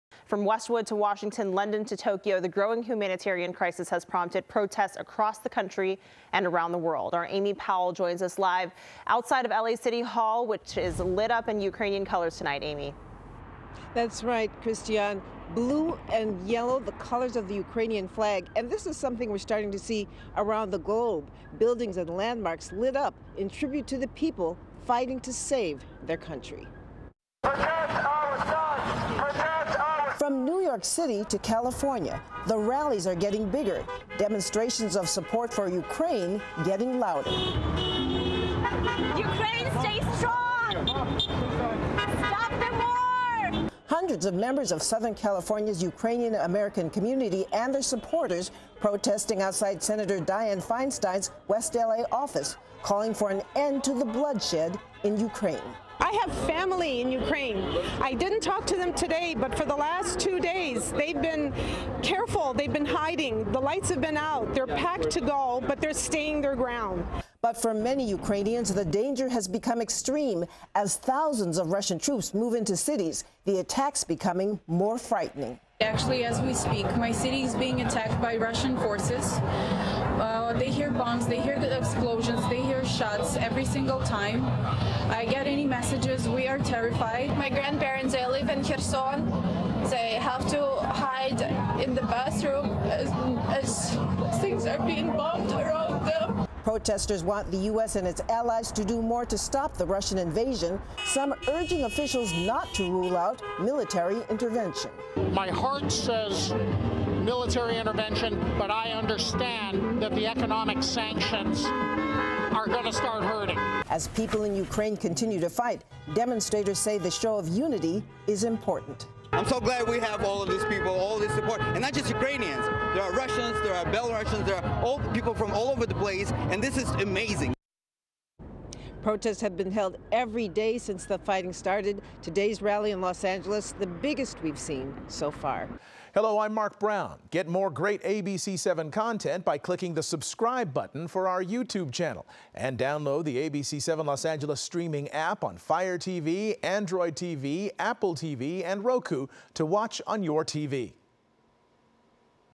Hundreds demonstrate for Ukraine in LA as worldwide support for the country continues to grow | ABC7
Hundreds of people gathered in the Westwood area of Los Angeles Saturday afternoon to demonstrate their support of Ukraine and demand the U.S. do more to assist the besieged country. MORE